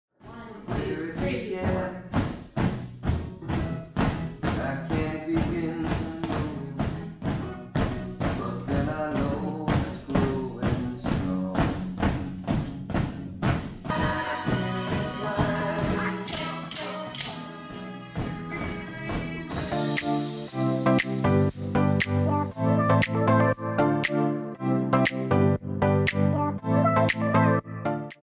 Trying out Drumba at Abilene Academy
Drumba, anyone?🥁 At Abilene Academy we get to see, do, and learn all the City has to offer -- including this popular class at Rose Park Senior Center! now through Nov. 30th.